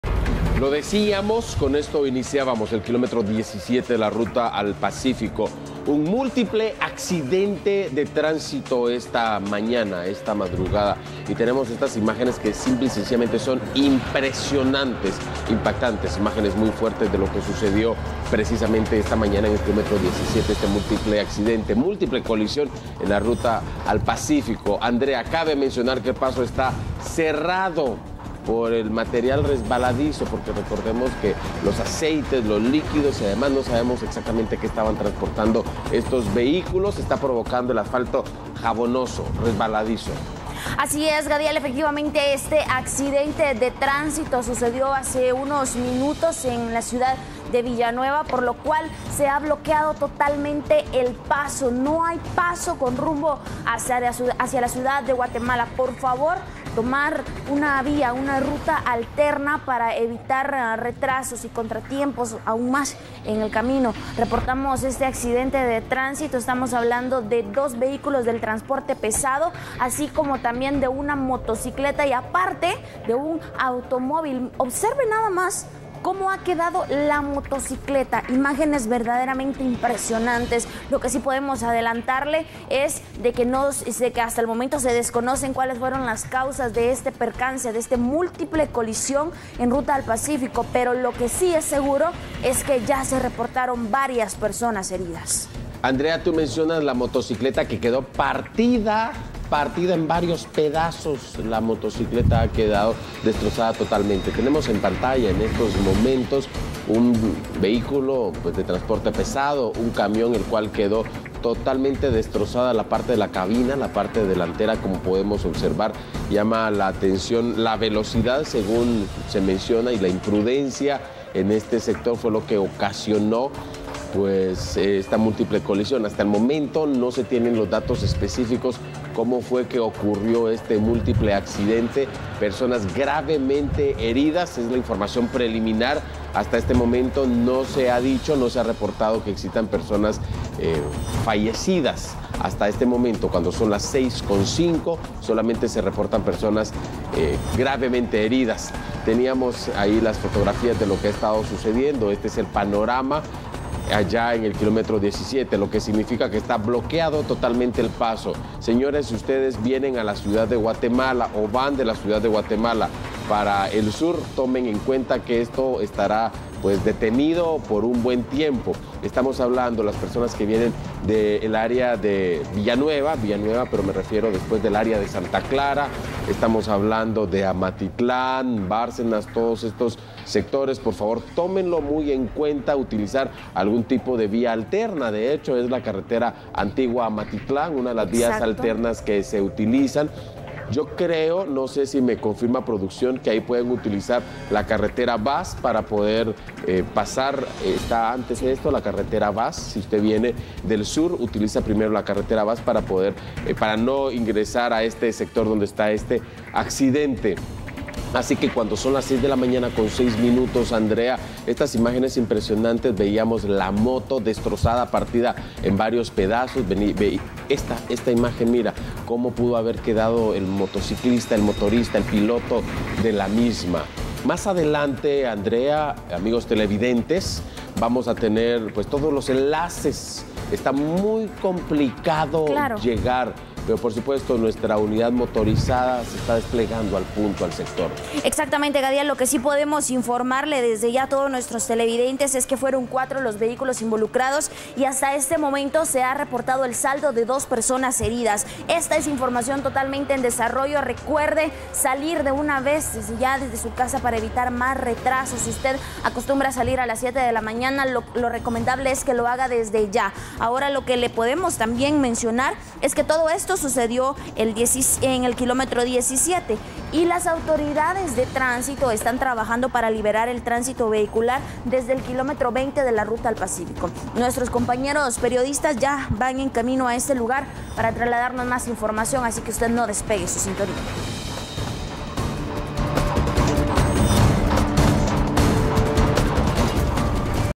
Múltiple accidente en la ruta al Pácifico
Un múltiple accidente se registró en la ruta al Pacífico, en el kilómetro 17. La colisión provocó consecuencias graves.